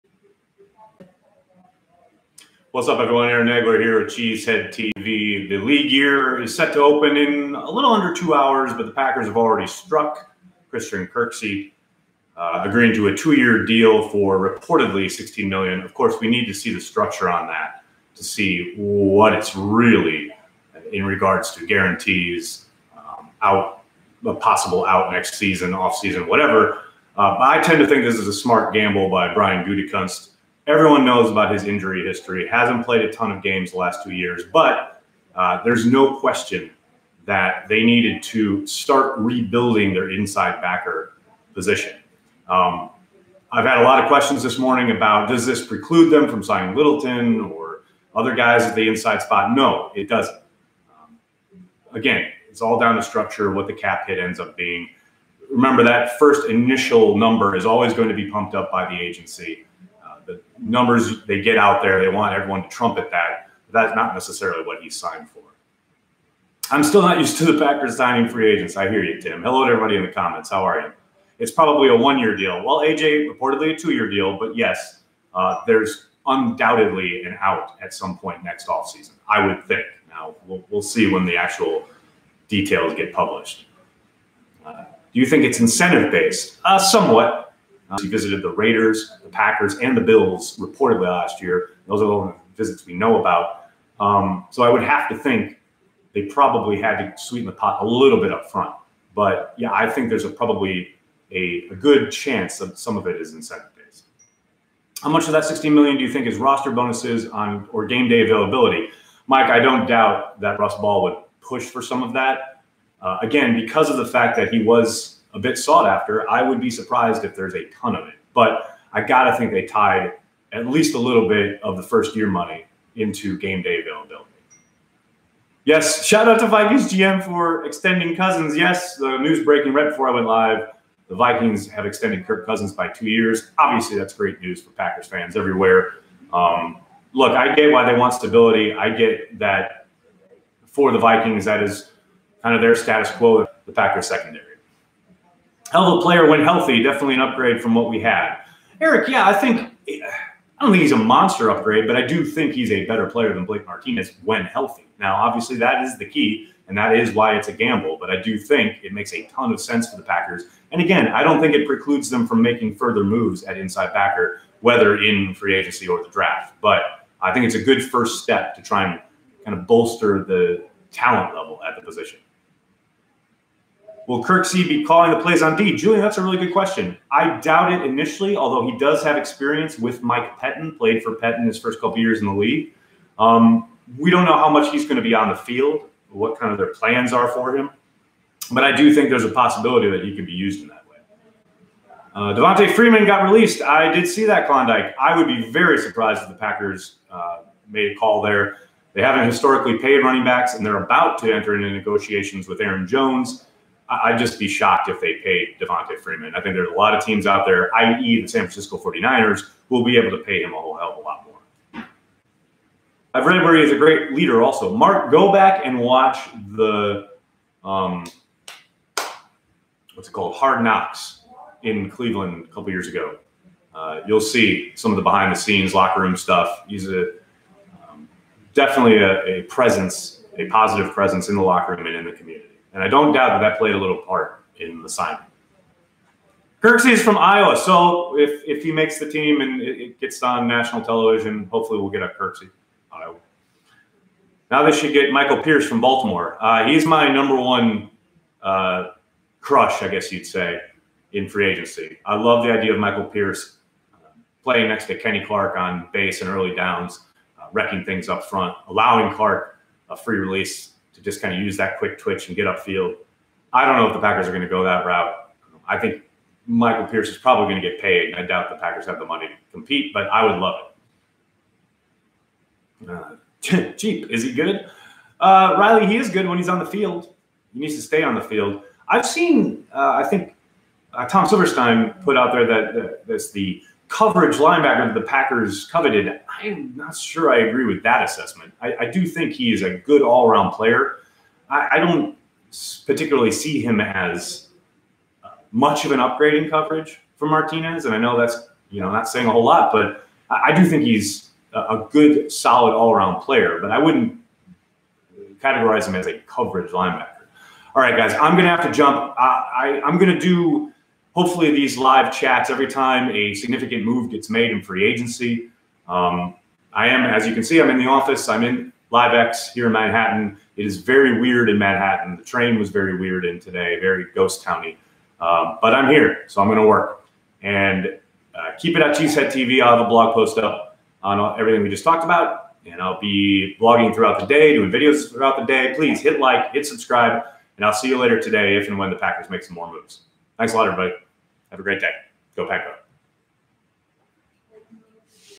Extra Cheese: Christian Kirksey smart gamble for the Packers
Aaron chats with Packers fans about the Christian Kirksey signing